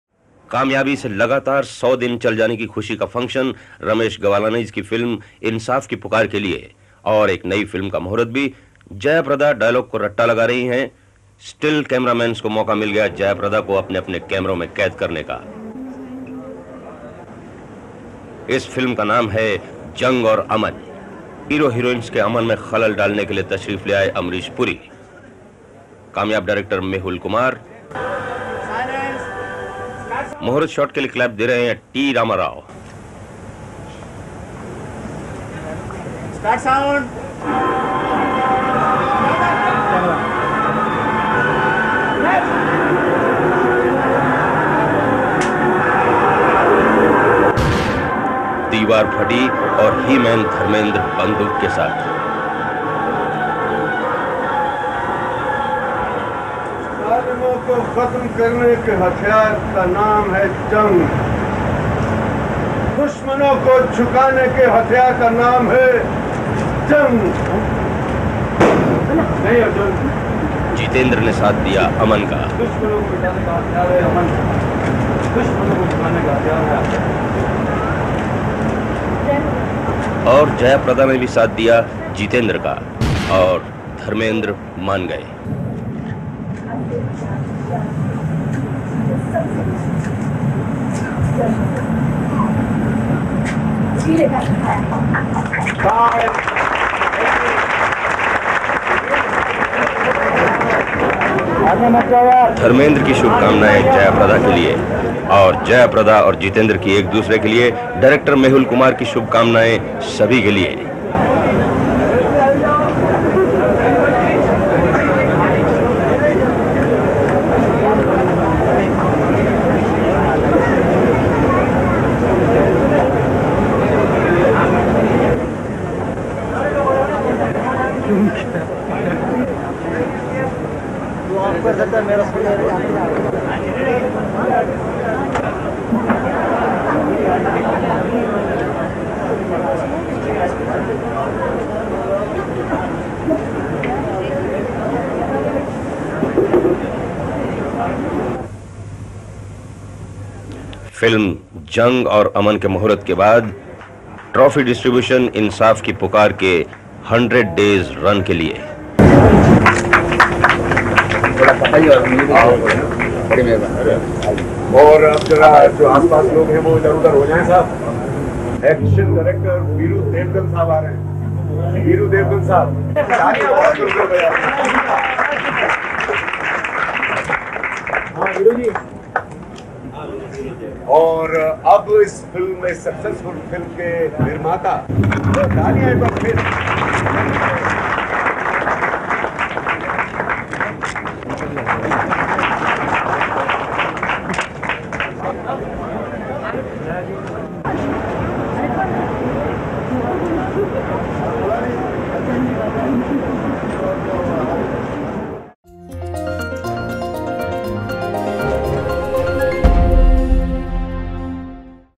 "Insaf Ki Pukar" Silver Jubilee Celebration & Muhurat Of "Jung Aur Aman"
Throwback from the silver jubliee celebration of "Insaf Ki Pukar" and muhurat of a film titled "Jung Aur Aman". "Insaf Ki Pukar", directed by T Rama Rao featured Dharmendra and Jeetendra. As the film successfully ran for 25 weeks in theatres, a celebratory event was organised where a new film featuring Dharmendra and Jeetendra was also announced. Titled "Jung Aur Aman", this action film had Jaya Prada as female lead. However, this film was never released.